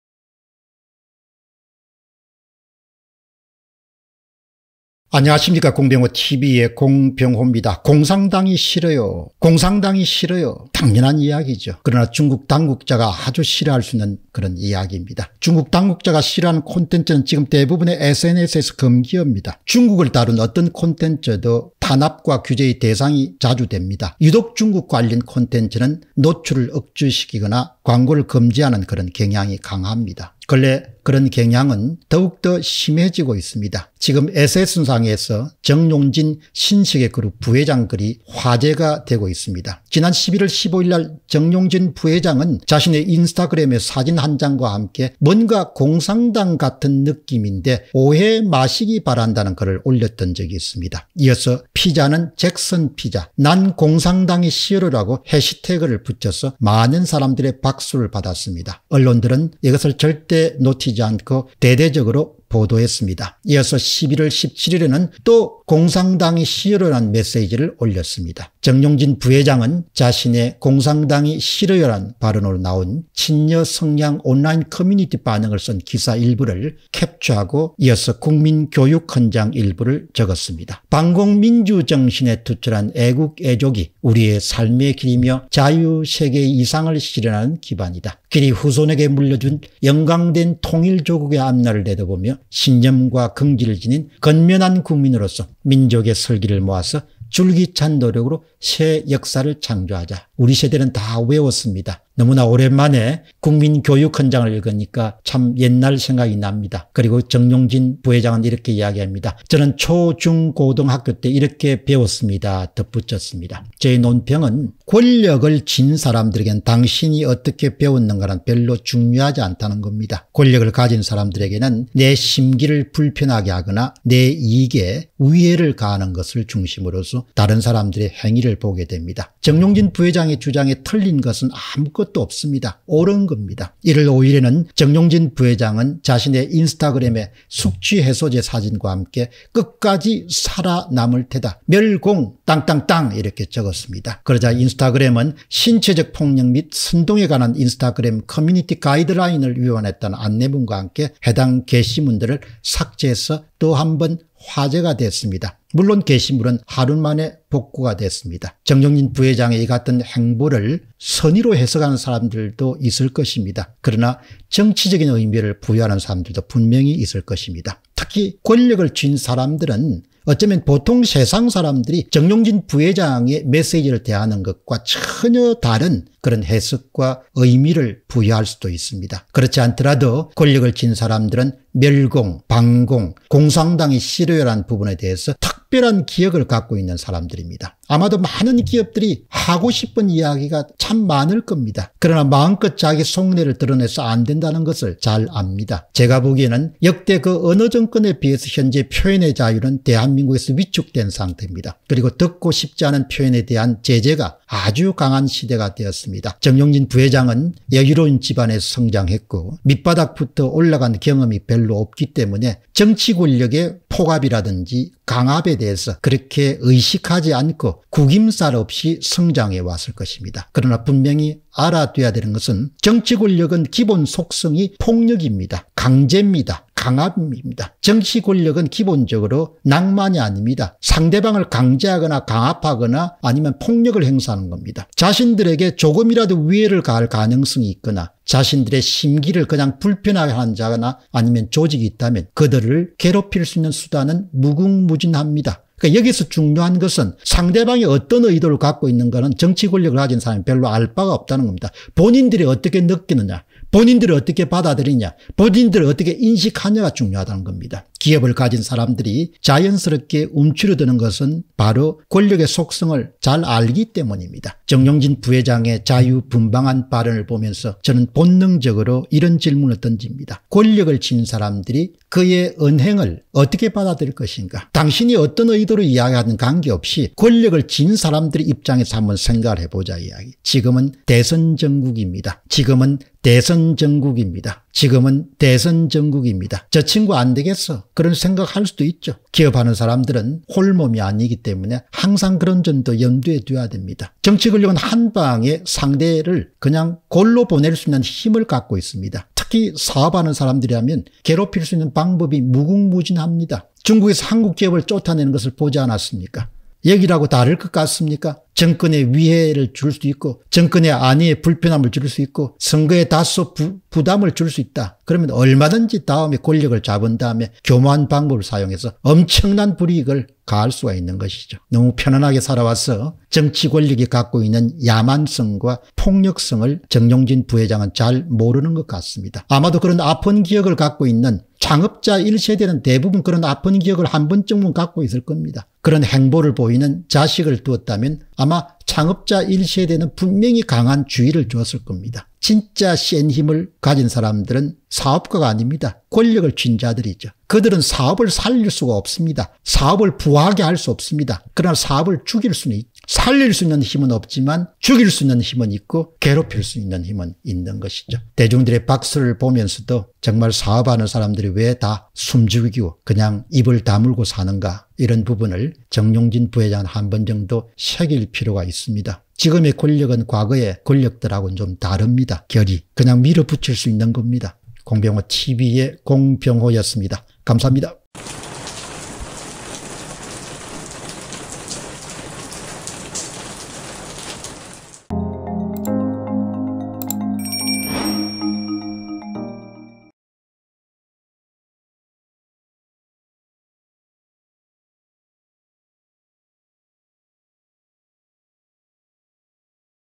정용진의 멸공 시리즈 / 옳은 말이지만, 권력을 쥔 자들의 생각은 다를 수 있어 / 정치권력이 불편하면 [공병호TV]
Bitcoin : 3MTW849AkP2ftLeSK2teZHMmzLu7Z83s9U